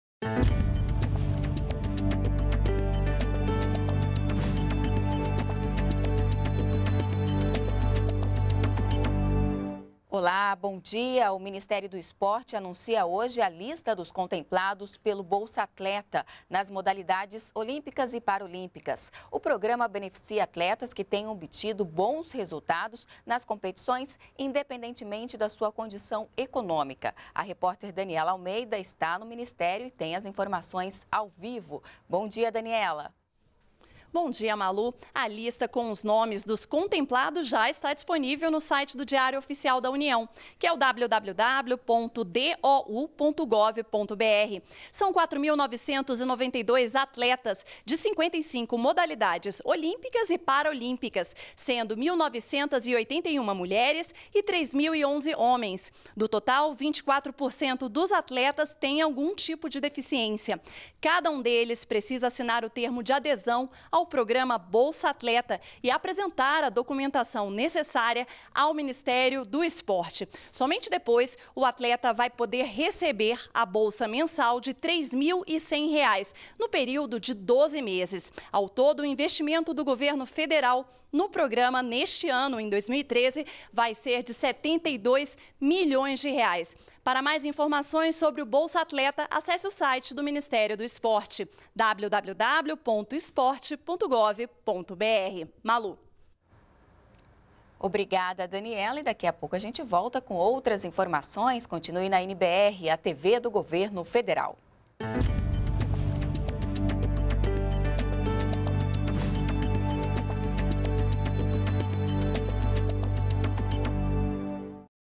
Lista de contemplados do Bolsa-Atleta será divulgada pelo Ministério do Esporte
ATIVIDADES DO GOVERNO - 15.02.13: O Ministério do Esporte anuncia, nesta sexta-feira (15), a lista dos contemplados pelo Bolsa-Atleta nas modalidades olímpicas e paraolímpicas. O programa beneficia atletas que tenham obtido bons resultados nas competições, independentemente da sua condição econômica. A repórter Daniella Almeida tem as informações.